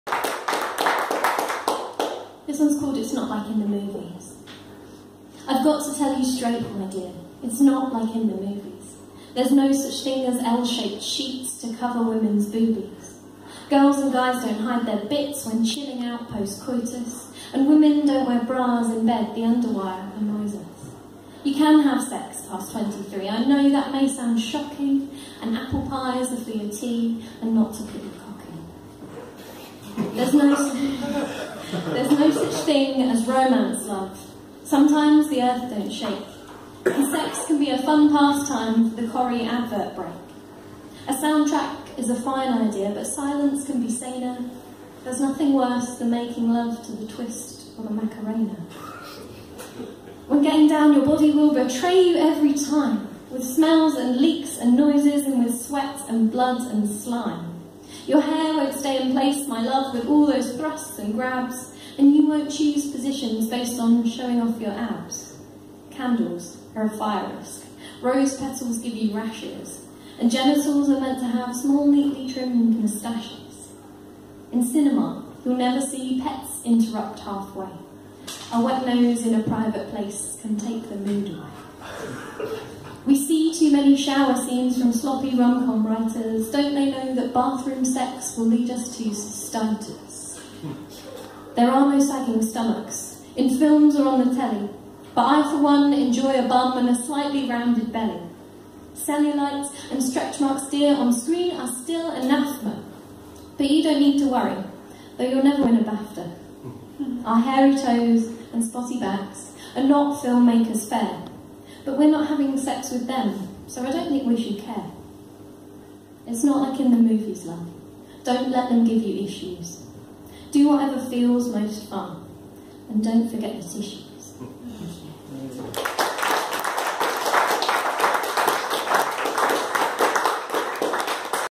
It's not like in the Movies
Leanne Moden performs 'It's not like in the Movies' at DIY Poets in Nottingham on 11th February 2016.